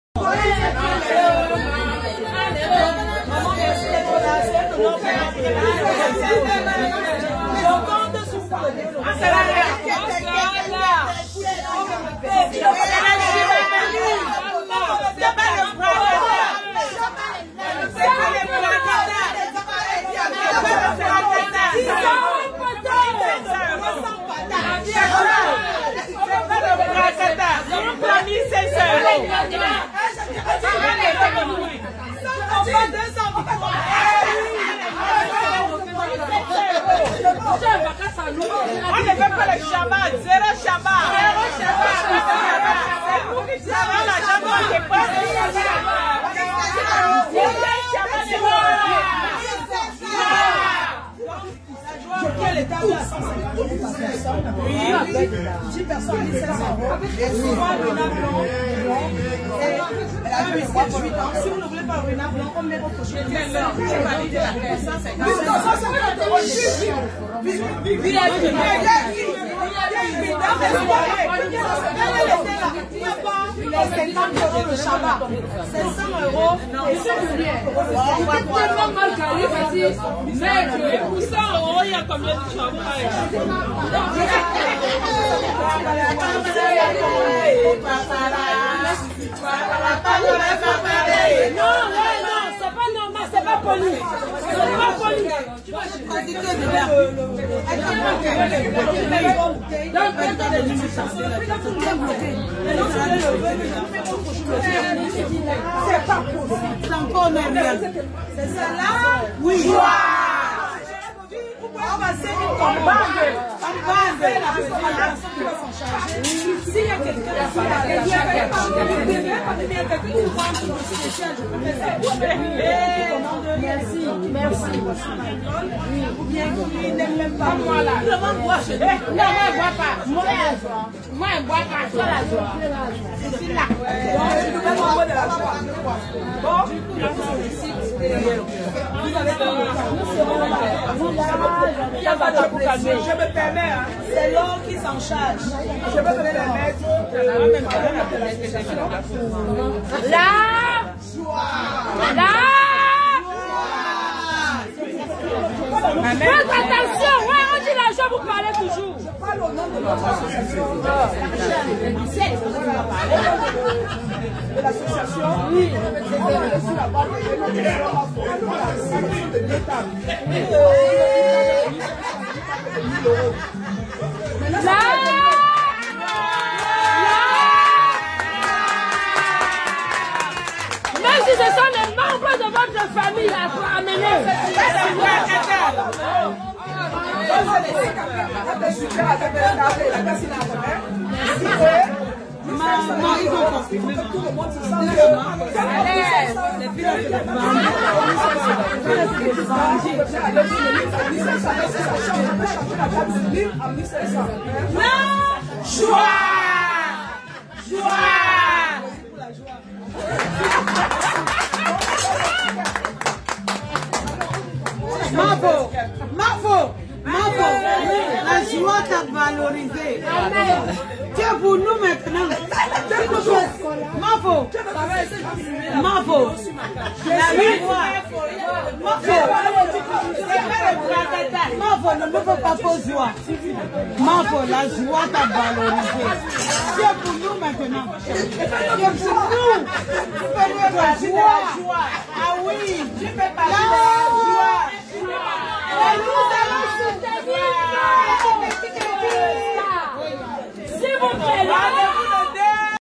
Flore de Lille est vraiment à féliciter et encourager, elle est sur le terrain pour la reussite...
Flore de Lille est vraiment à féliciter et encourager, elle est sur le terrain pour la reussite de sa soirée